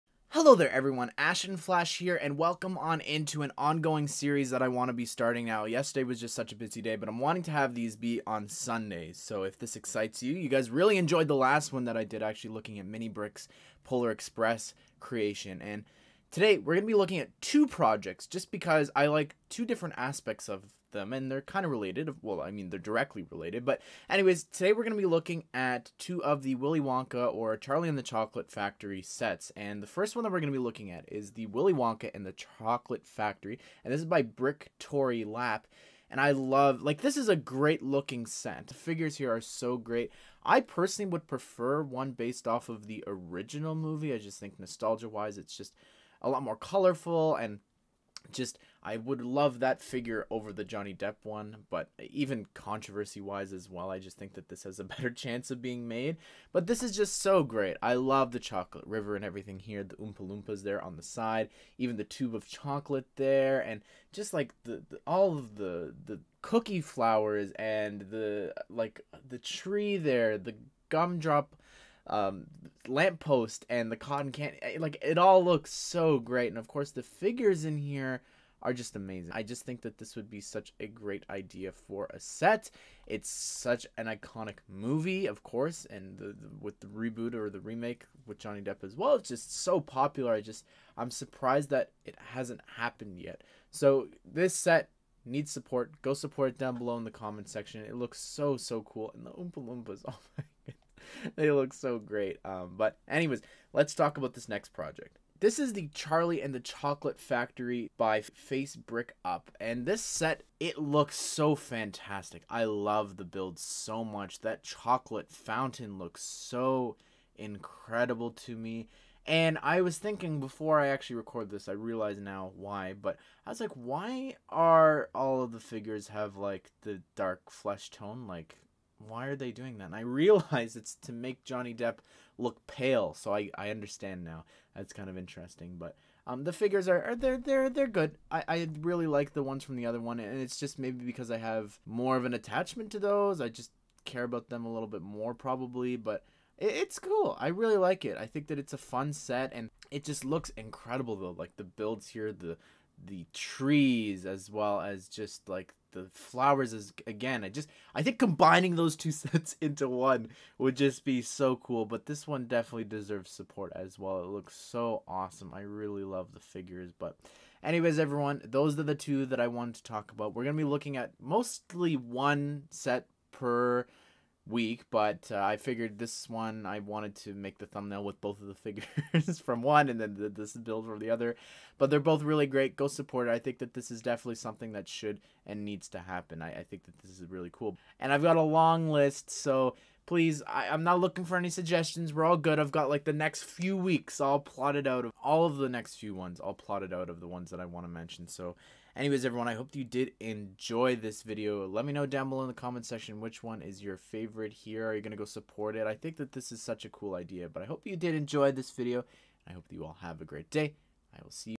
LEGO IDEAS Willy Wonka Project Showcases
Today I'm sharing TWO projects for Willy Wonka and the Chocolate Factory!